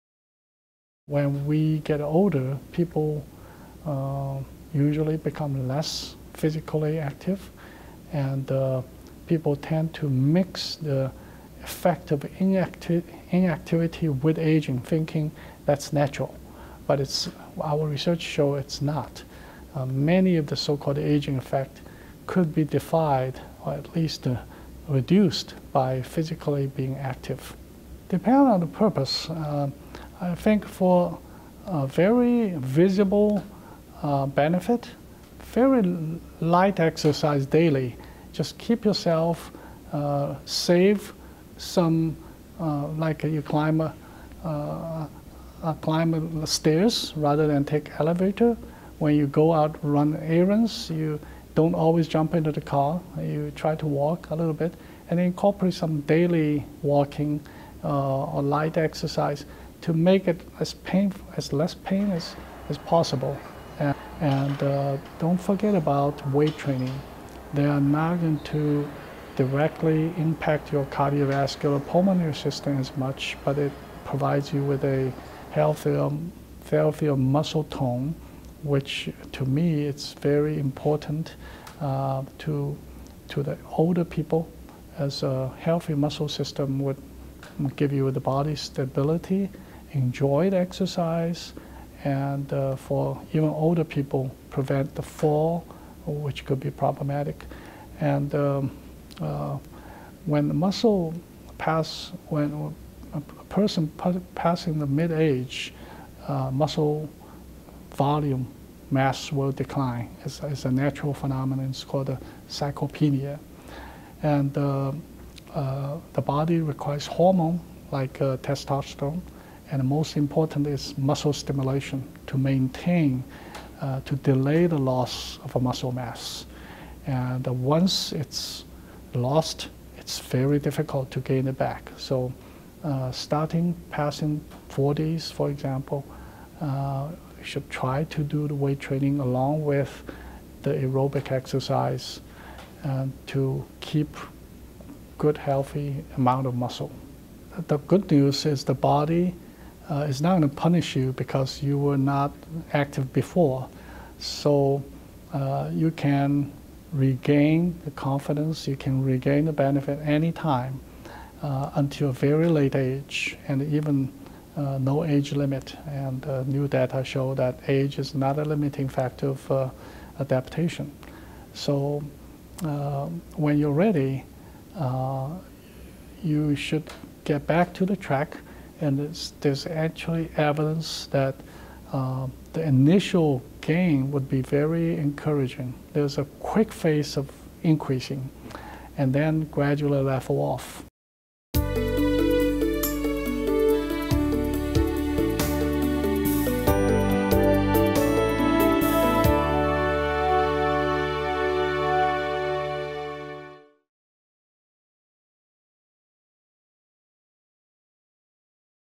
Li Li Ji
Exercise and Bones Strength - Li Li Ji, PhD, University of Minnesota, Minneapolis, MN